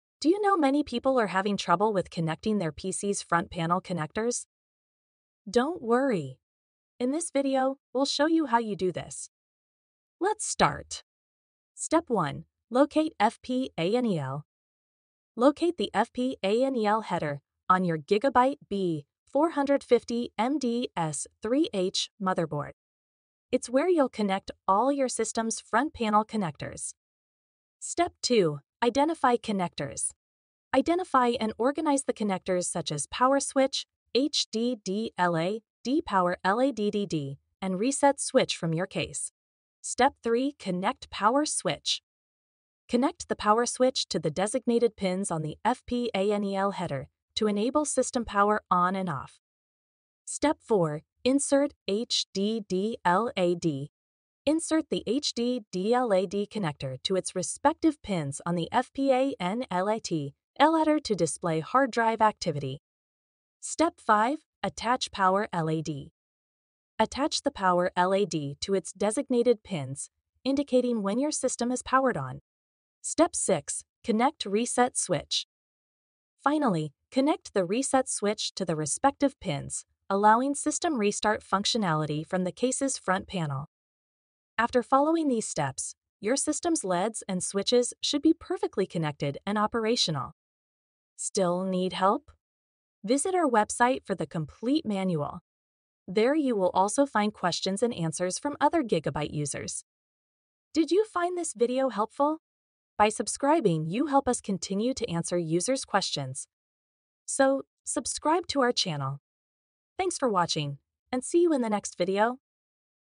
How to Connect LEDs and Switches on your Gigabyte B450M DS3H Motherboard
📺 Learn how to connect LEDs and switches on your Gigabyte B450M DS3H motherboard. Watch this video to master front panel connectors!

📖 In 6 steps:
1. Locate the F_PANEL header on your Gigabyte B450M DS3H motherboard. It's where you'll connect all your system's front panel connectors.
2. Identify and organize the connectors such as Power Switch, HDD LED, Power LED, and Reset Switch from your case.
3. Connect the Power Switch to the designated pins on the F_PANEL header to enable system power on and off.
4. Insert the HDD LED connector to its respective pins on the F_PANEL header to display hard drive activity.
5. Attach the Power LED to its designated pins, indicating when your system is powered on.
6. Finally, connect the Reset Switch to the respective pins, allowing system restart functionality from the case's front panel.

📚💬 Manual and other questions:
On our site, you can find the complete manual for the Gigabyte B450M DS3H. There, you can also ask your question to other Gigabyte B450M DS3H users.